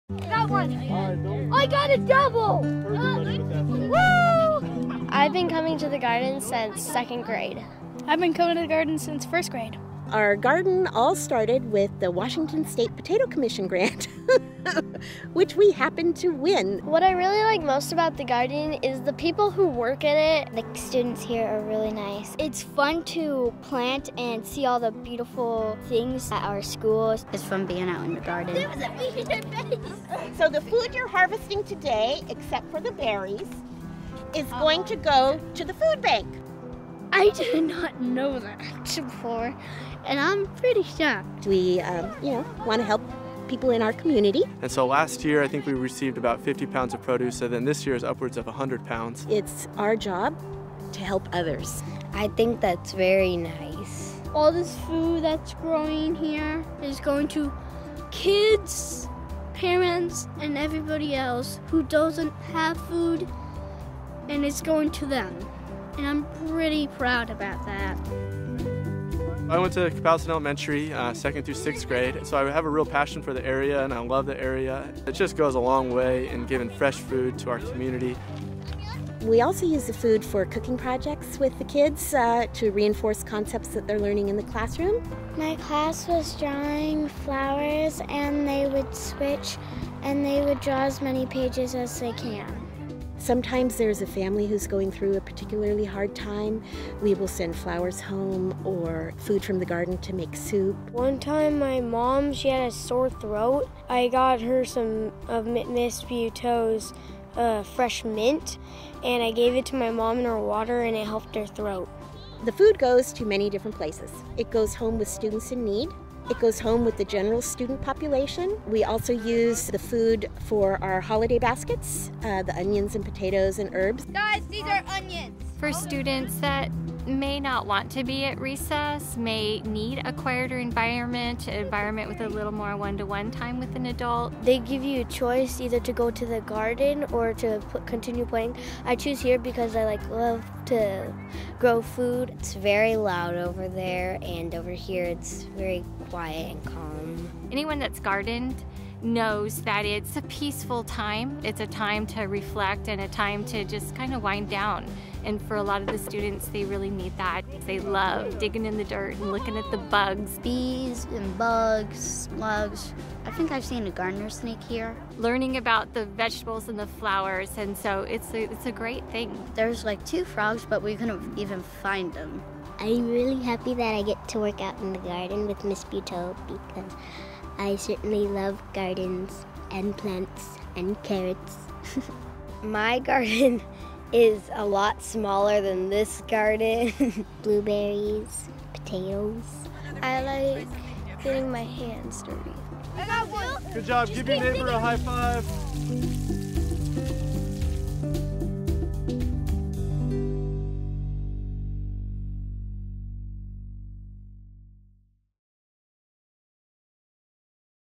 Kapowsin Elementary Community Garden
Students at Kapowsin Elementary love getting their hands dirty in the school's community garden. The fruits, vegetables and flowers grown on campus are used in many ways, one of which is as donations for the local food bank.